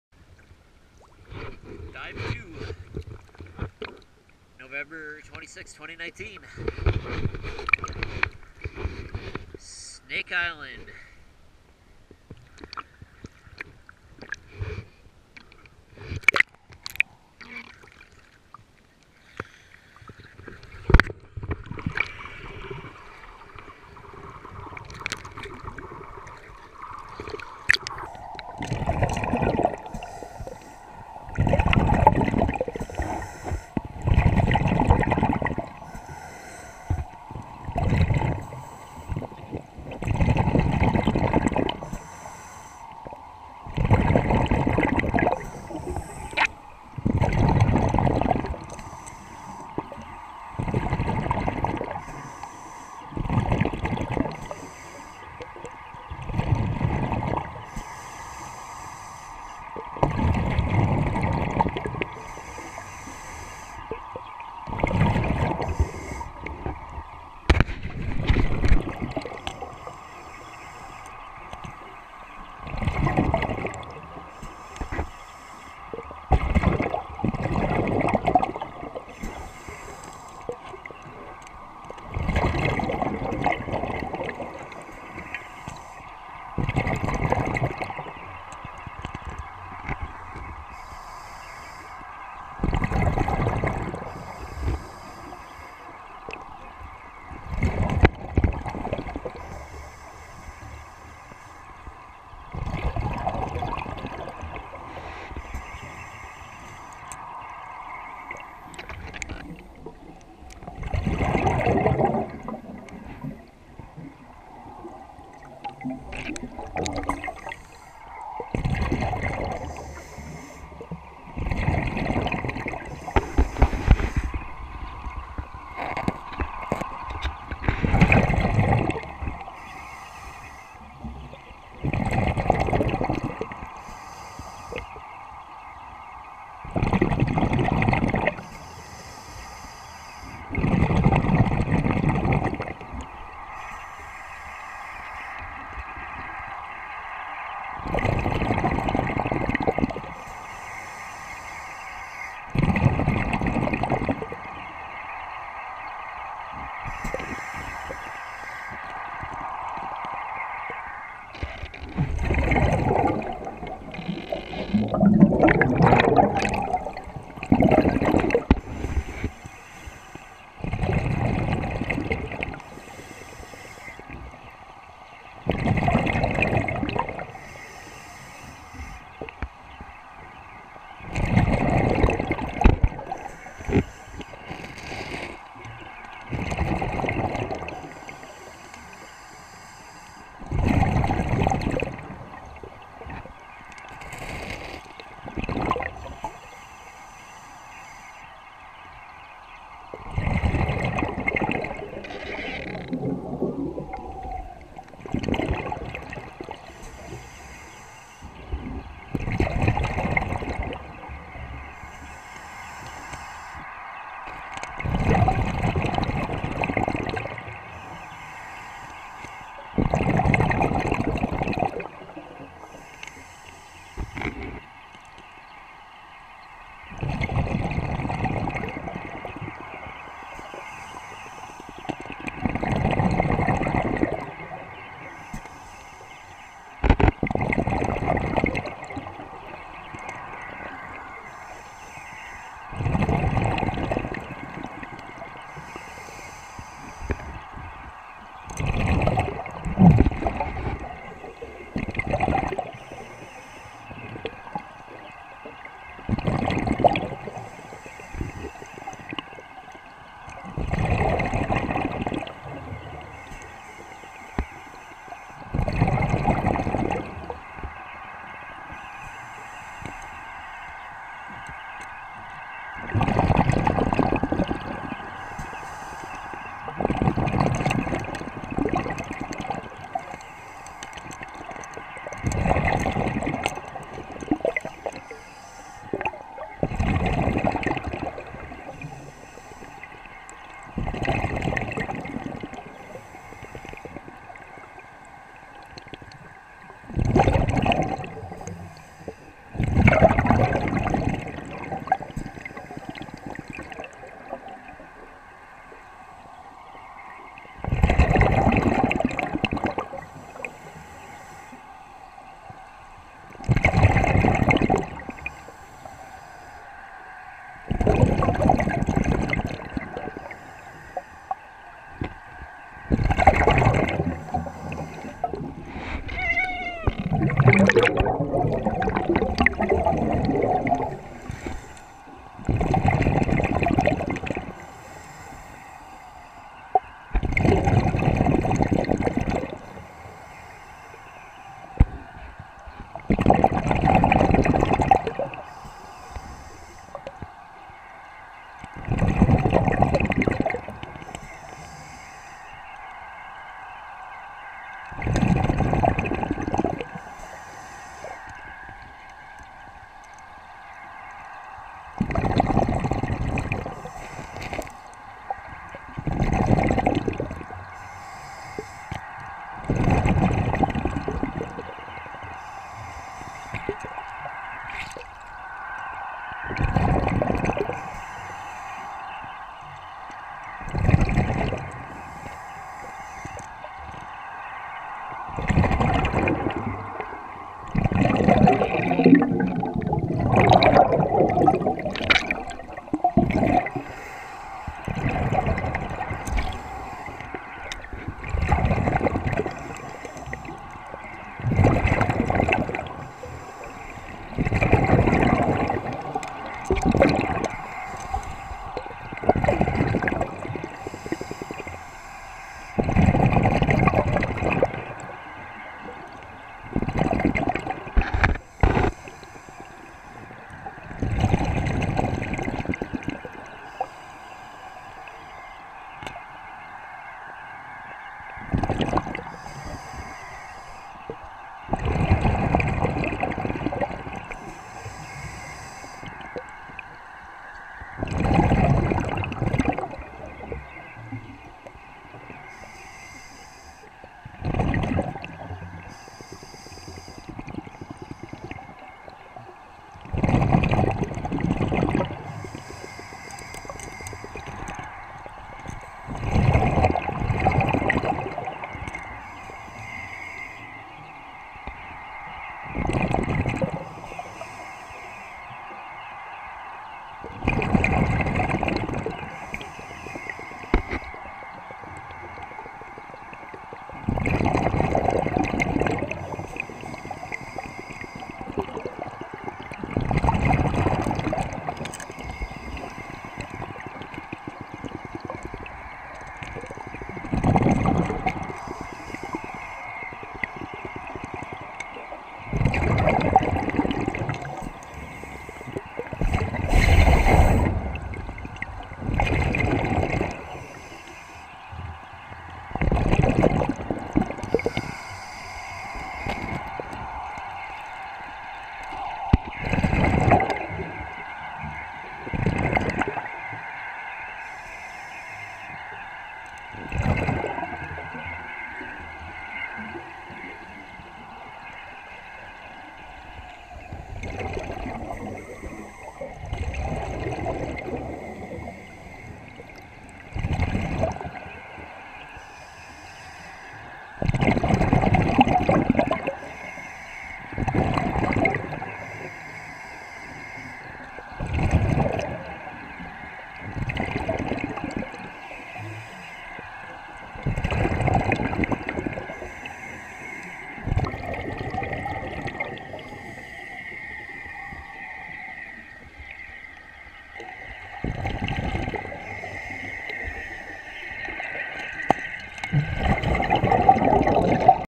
Snake Island dive 2 cam 2
2nd exploratory dive for whitefish eggs off Snake Island, Green Bay Lake Michigan, November 26, 2019.